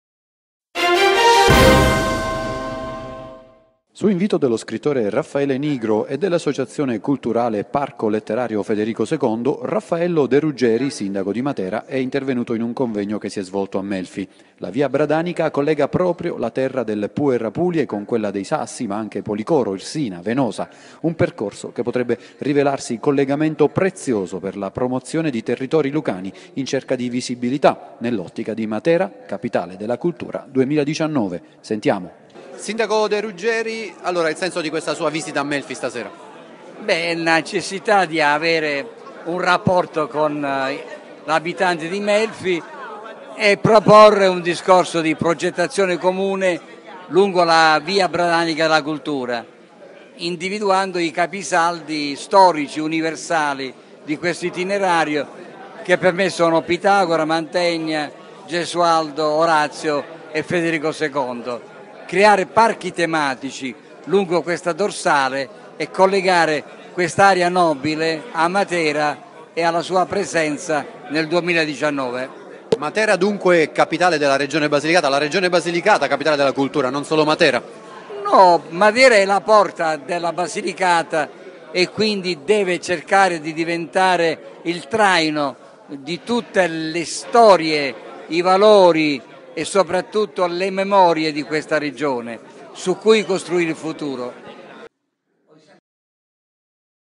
MELFI | Il sindaco di Matera a Melfi
De Ruggieri a Melfi parla della via Bradanica